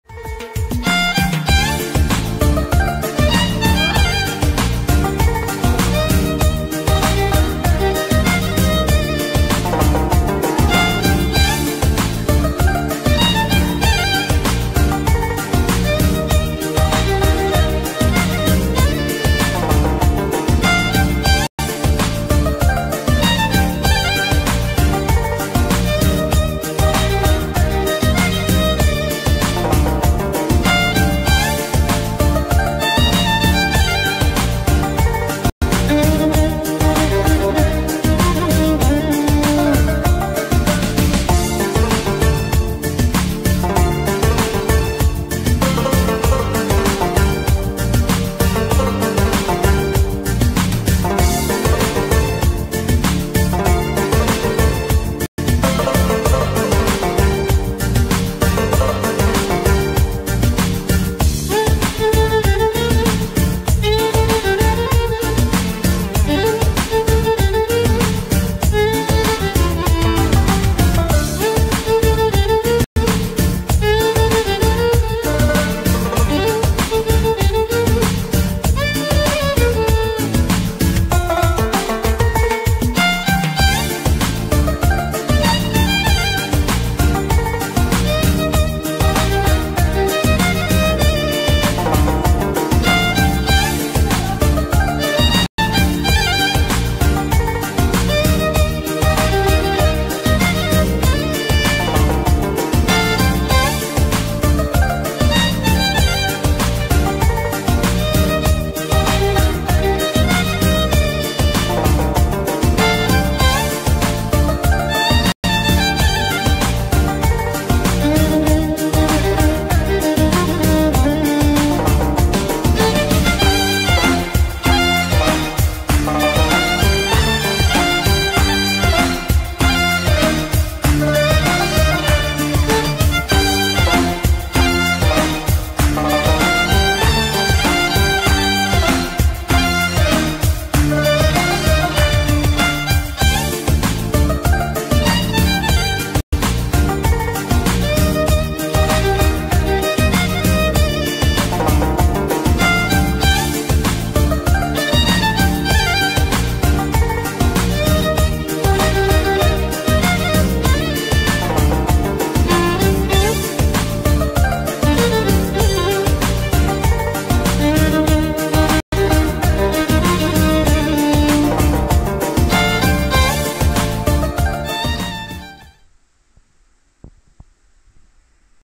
المخرج حسين العوضي - معرض - مقتنيات - طائرات
جولة في معرضي الشخصي للمقتنيات والطائرات وبعض من الذكريات